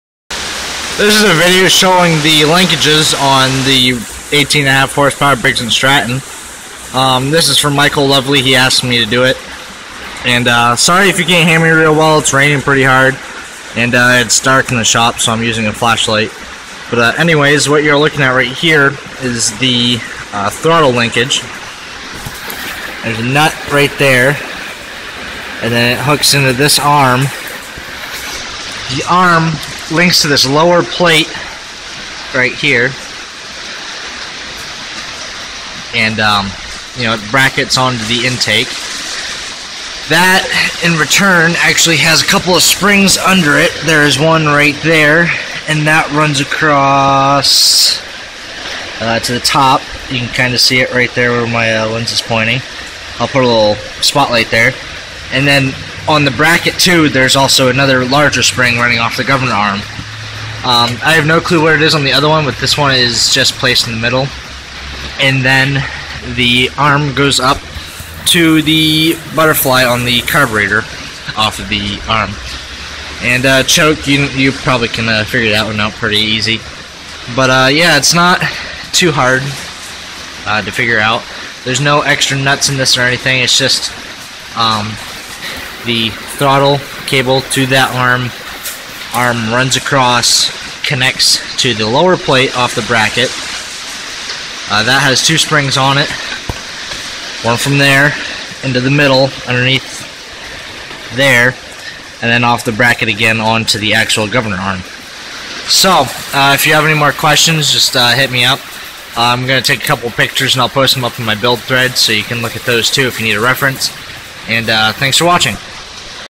Briggs and Stratton 18hp Opposed Twin (Flathead) Throttle Linkages (420000 series)
Throttle linkage video for Micheal Lovely. Sorry for being so loud, phone didnt pick up the background noise as much as I thought it did.